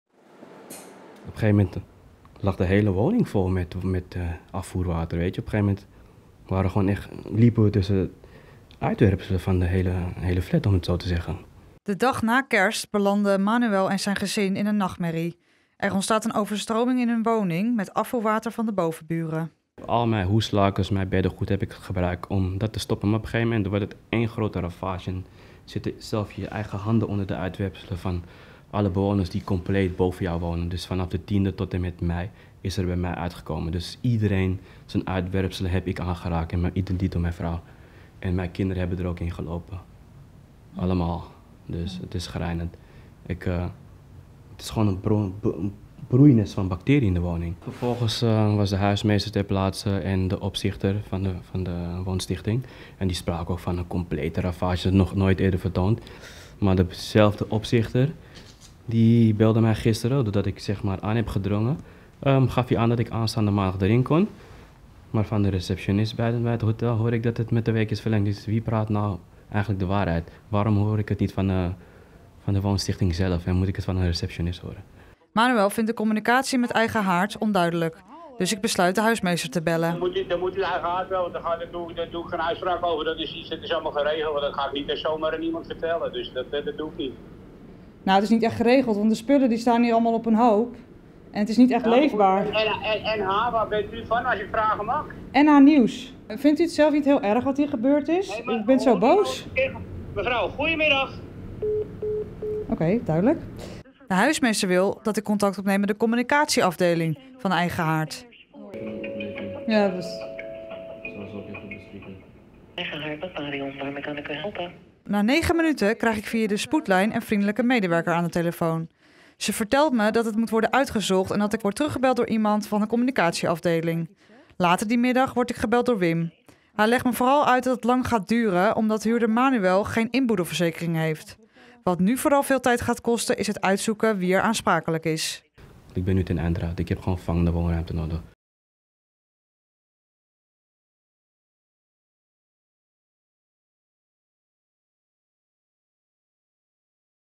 Manuels appartement overstroomd met 'schijtwater' van bovenburen
Uithoornaar Manuel Tan A Kiam is radeloos: door een probleem met de riolering overstroomde in de laatste week van vorig jaar zijn appartement op de begane grond van de Coudenhoveflat in Uithoorn met 'schijtwater' van al zijn buren op de negen etages boven hem. "Ik heb ieders uitwerpselen in mijn handen gehad, en mijn vrouw ook."